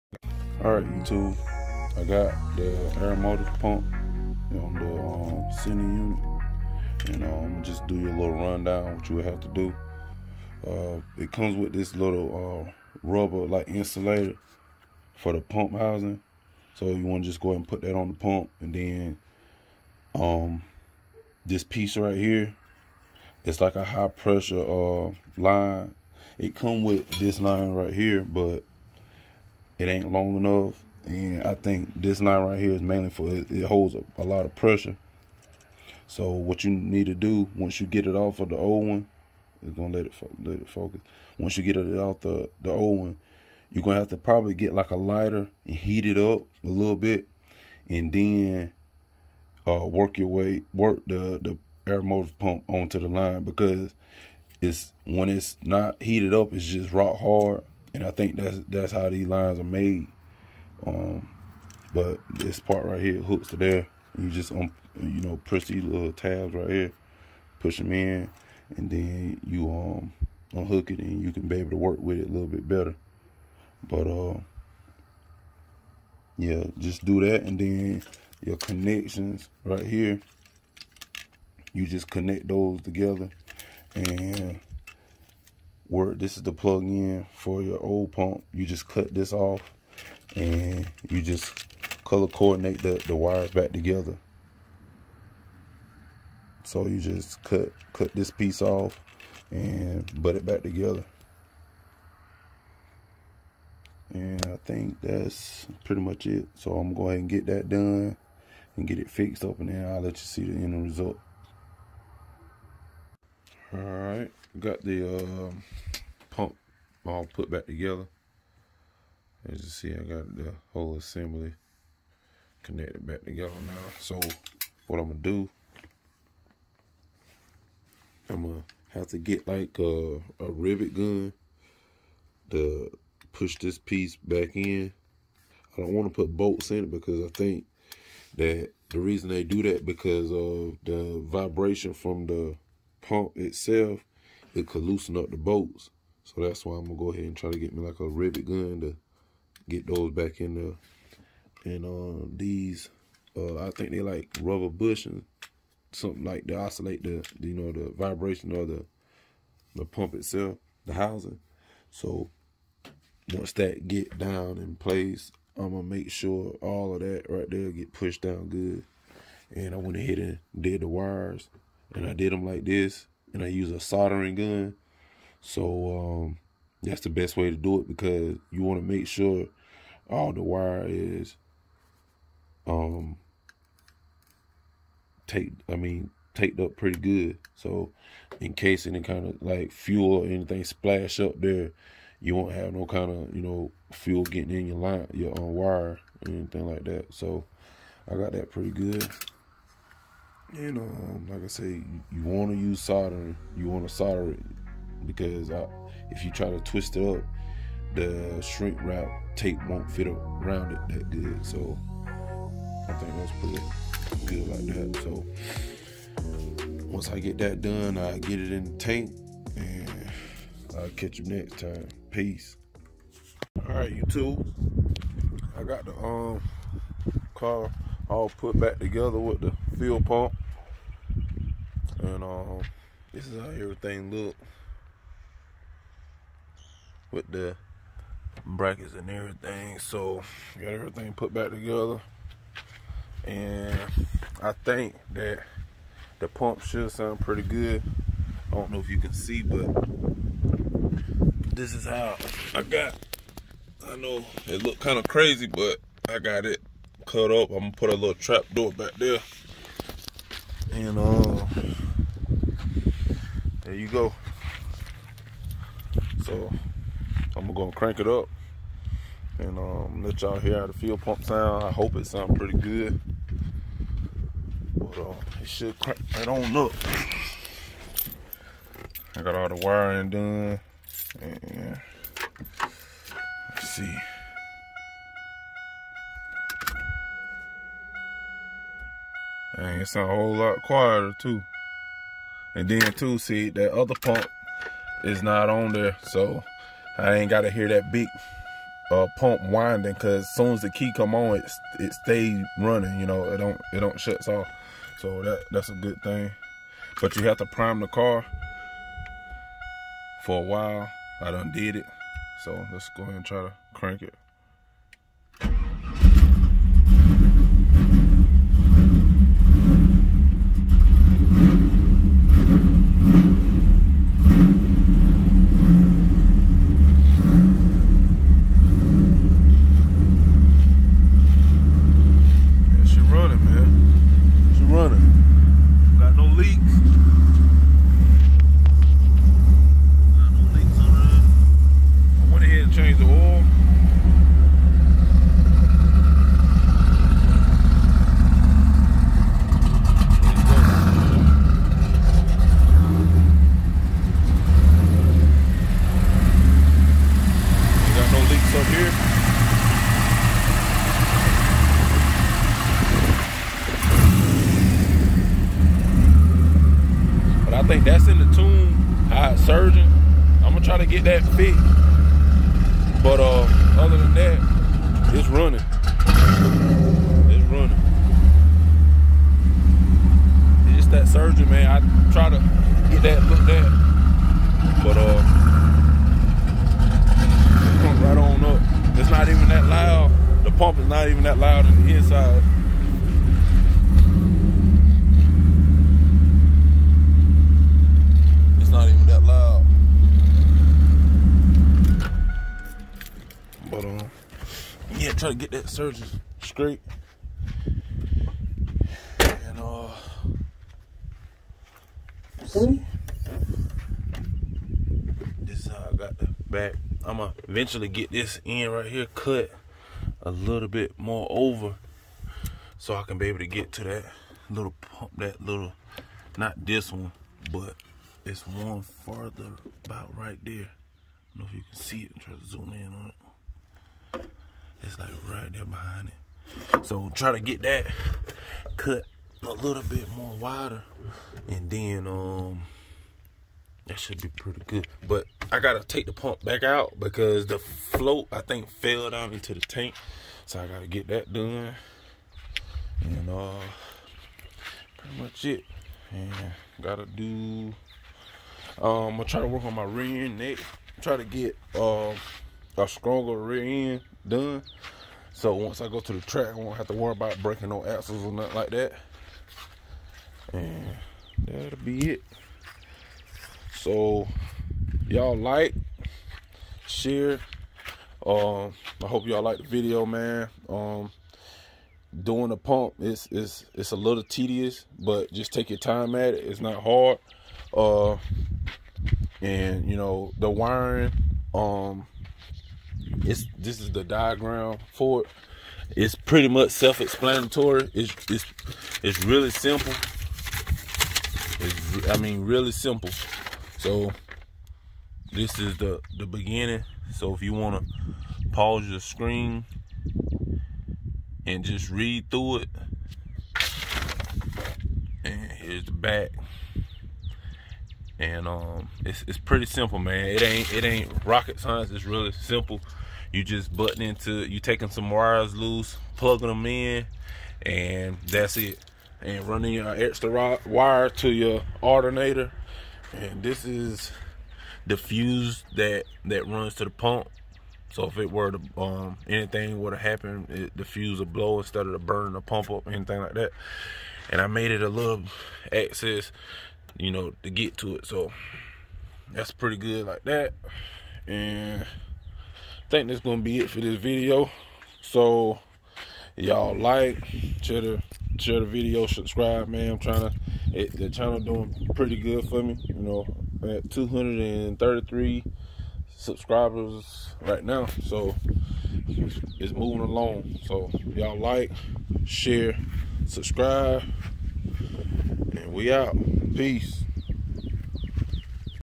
My F-body 340lph Fuel Pump Upgrade
I orderd the Aeromotive fuel pump from Texas Speed Performance part #11569